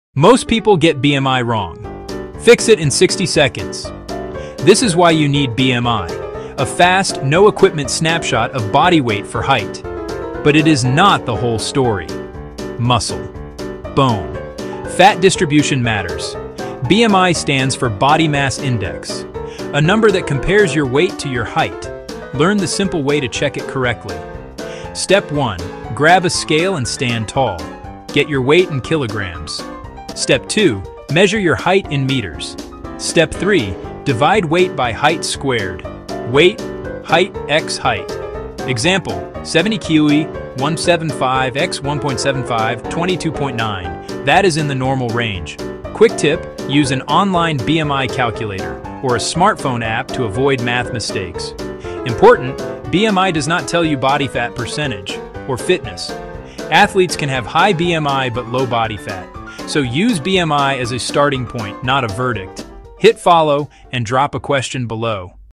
You’ve Been Measuring BMI Wrong All Along 😱 Here’s the Truth
Most people get their BMI (Body Mass Index) wrong!
In this quick 60-second explainer, learn how to calculate your BMI correctly and understand what it really means for your health.

✅ Step-by-step guide:

1️⃣ Measure your height (in meters) and weight (in kilograms).
2️⃣ Use the formula: BMI = weight ÷ (height × height) or a reliable BMI calculator.
3️⃣ Check your range — underweight, normal, overweight, or obese.
4️⃣ Know the limits — BMI doesn’t consider age, muscle, or ethnicity.

Perfect for quick health awareness and smart weight management!